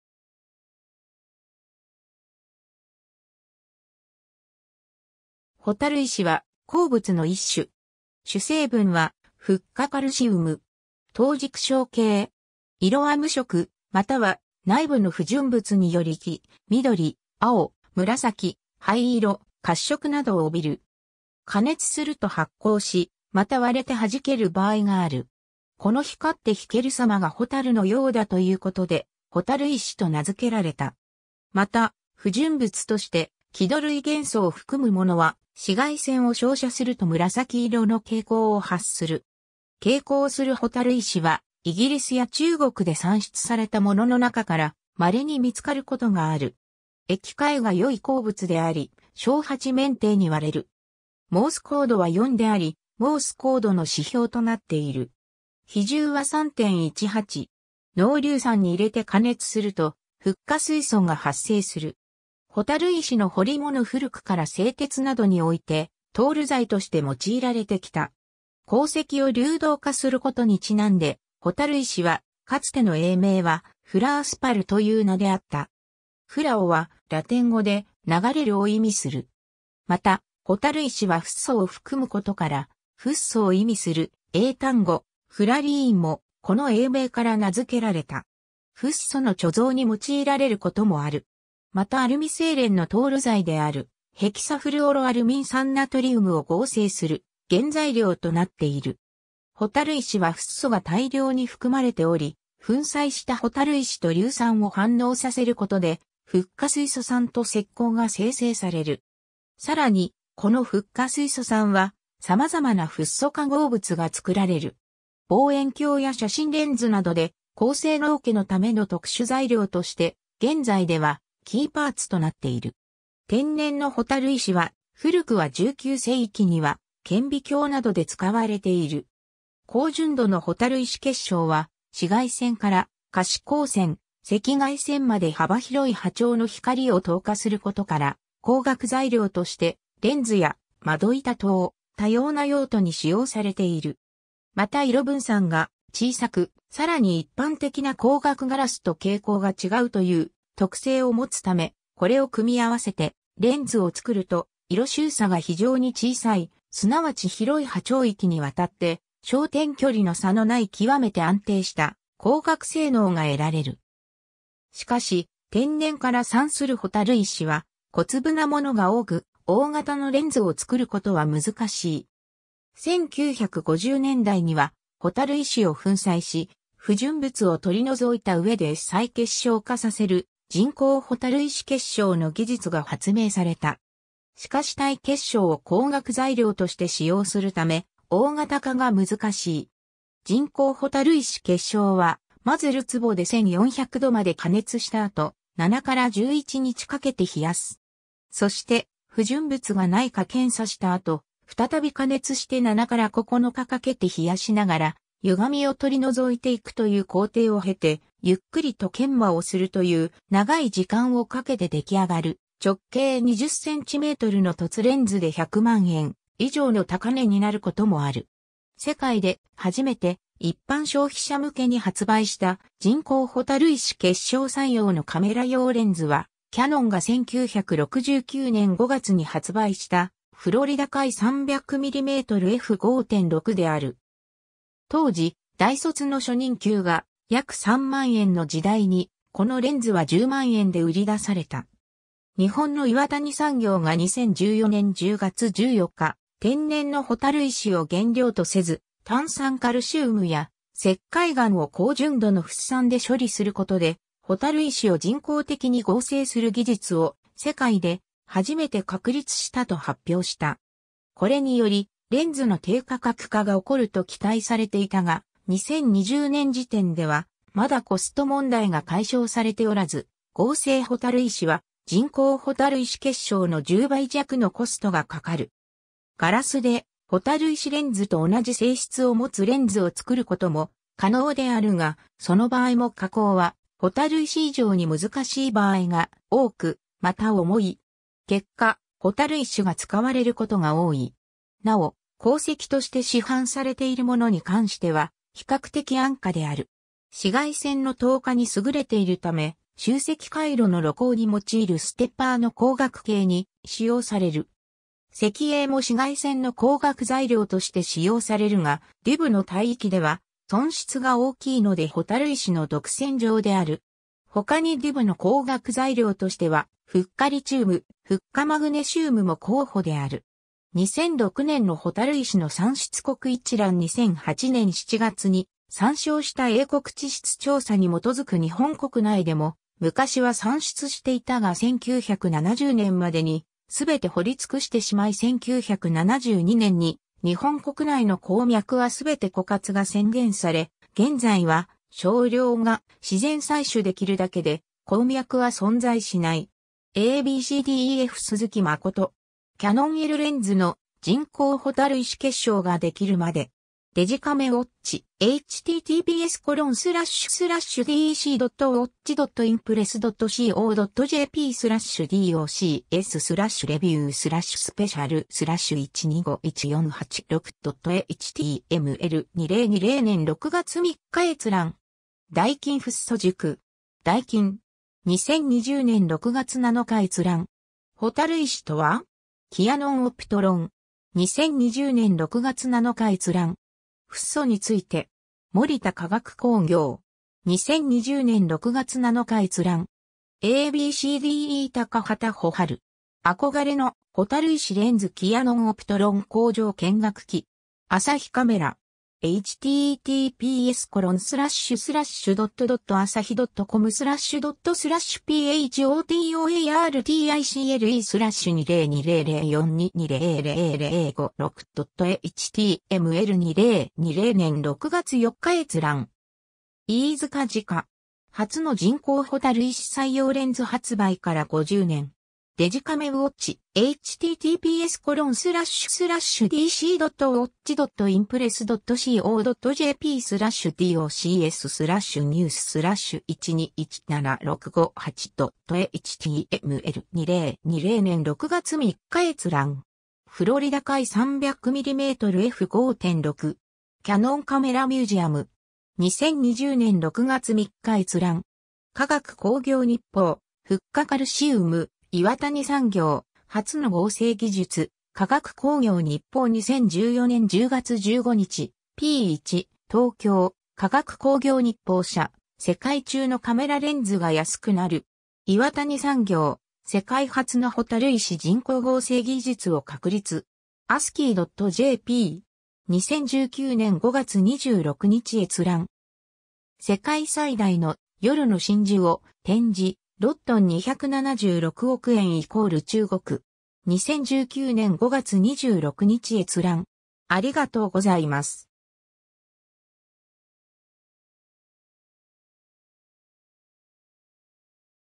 蛍石
蛍石（ほたるいし/けいせき、螢石、Fluorite、フローライト、フルオライト）は、鉱物（ハロゲン化鉱物）の一種。
主成分はフッ化カルシウム（CaF2）。
等軸晶系。
 色は無色、または内部の不純物により黄、緑、青、紫、灰色、褐色などを帯びる。
加熱すると発光し、また割れてはじける場合がある。
この光って弾ける様が蛍のようだということで、蛍石と名付けられた。
また、不純物として希土類元素を含むものは、紫外線を照射すると紫色の蛍光を発する。
蛍光する蛍石はイギリスや中国で産出されたものの中から稀に見つかることがある。
 へき開が良い鉱物であり、正八面体に割れる。
モース硬度は4であり、モース硬度の指標となっている。
比重は3.18。
濃硫酸に入れて加熱するとフッ化水素が発生する。
 蛍石の彫り物 古くから製鉄などにおいて融剤として用いられてきた。
鉱石を流動化することにちなんで、蛍石はかつての英名は「fluorspar」という名であった。
fluoはラテン語で「流れる」を意味する。
また、蛍石はフッ素を含むことから、フッ素を意味する英単語「fluorine」も、この英名から名付けられた。
 フッ素の貯蔵に用いられることもある。
またアルミ精錬の融剤であるヘキサフルオロアルミン酸ナトリウムを合成する原材料となっている。
 蛍石はフッ素が大量に含まれており、粉砕した蛍石と硫酸を反応させることで、フッ化水素酸と石膏が生成される。
さらに、このフッ化水素酸は、様々なフッ素化合物が作られる。
 望遠鏡や写真レンズ（特に望遠レンズ）などで、高性能化のための特殊材料として現在ではキーパーツとなっている。
天然の蛍石は、古くは19世紀には、顕微鏡などで使われている。
 高純度の蛍石結晶は、紫外線から可視光線、赤外線まで幅広い波長の光（130nmから8μm）を透過することから、光学材料としてレンズや窓板等、多様な用途に使用されている。
また色分散が小さく、さらに一般的な光学ガラスと傾向が違う（異常部分分散）という特性を持つため、これを組み合わせてレンズを作ると色収差が非常に小さい、すなわち広い波長域にわたって焦点距離の差のない極めて安定した光学性能が得られる（蛍石レンズ）。
 しかし、天然から産する蛍石は小粒なものが多く、大型のレンズを作ることは難しい。
1950年代には、蛍石を粉砕し、不純物を取り除いた上で再結晶化させる人工蛍石結晶の技術が発明された。
しかし単結晶を光学材料として使用するため、大型化が難しい。
人工蛍石結晶は、まず坩堝で1400度まで加熱したあと、7～11日かけて冷やす。
そして、不純物がないか検査した後、再び加熱して7～9日かけて冷やしながら、歪みを取り除いていくという工程を経て、ゆっくりと研磨をするという長い時間をかけて出来上がる、直径20cmの凸レンズで100万円以上の高値になることもある。
世界で初めて、一般消費者向けに発売した人工蛍石結晶採用のカメラ用レンズは、キヤノンが1969年5月に発売した「FL-F300mm F5.6」である。
当時、大卒の初任給が約3万円の時代に、このレンズは10万円で売り出された。
 日本の岩谷産業が2014年10月14日、天然の蛍石を原料とせず、炭酸カルシウムや石灰岩を高純度のフッ酸で処理することで、蛍石を人工的に合成する技術を、世界で初めて確立したと発表した。
これにより、レンズの低価格化が起こると期待されていたが、2020年時点ではまだコスト問題が解消されておらず、合成蛍石は、人工蛍石結晶の10倍弱のコストがかかる。
ガラスで、蛍石レンズと同じ性質を持つレンズを作ることも可能であるが、その場合も加工は蛍石以上に難しい場合が多く、また重い。
結果、蛍石が使われることが多い。
 なお、鉱石として市販されている物に関しては比較的安価である。
 紫外線の透過に優れているため、集積回路の露光に用いるステッパーの光学系に使用される。
石英も紫外線の光学材料として使用されるが、DUV(Deep Ultraviolet:深紫外線)の帯域では損失が大きいので蛍石の独擅場である。
他にDUVの光学材料としてはフッ化リチウム、フッ化マグネシウムも候補である。
 2006年の蛍石の産出国一覧 2008年7月に参照した英国地質調査に基づく 日本国内でも昔は産出していたが1970年までに全て掘りつくしてしまい1972年に日本国内の鉱脈は全て枯渇が宣言され現...